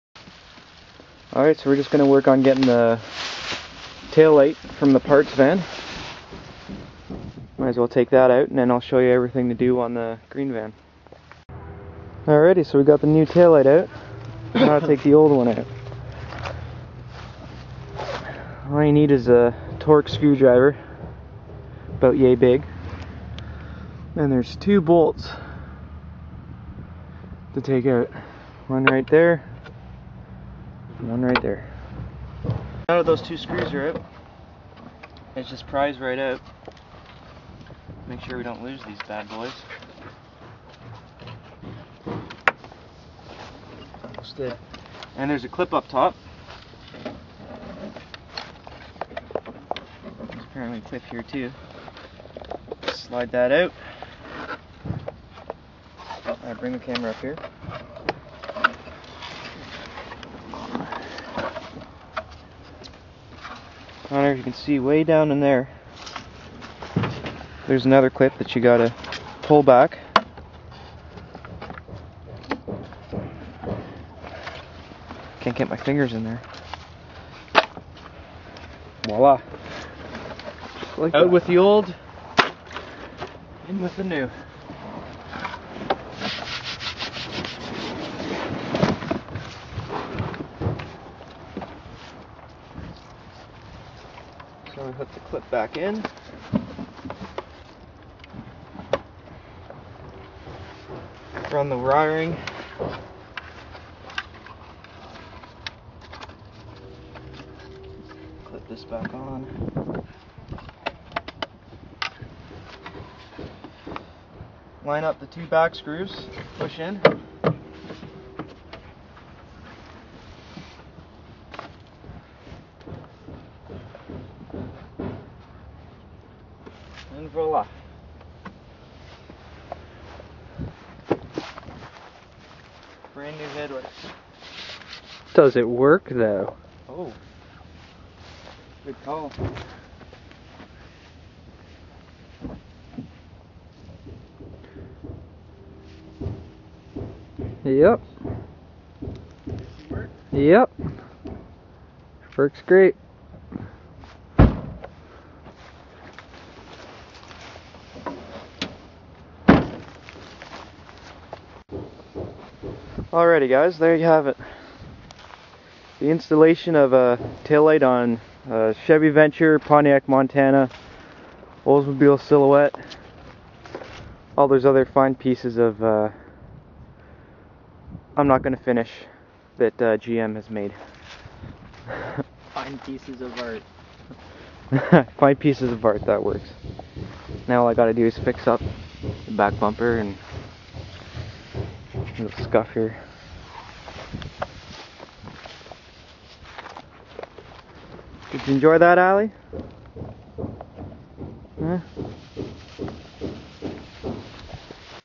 How To: 1997-2005 GM Mini Van Tail Light Replacement
A quick video of how to replace a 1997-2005 Chevrolet Venture, Pontiac Montana and Oldsmobile Sillouette tail light.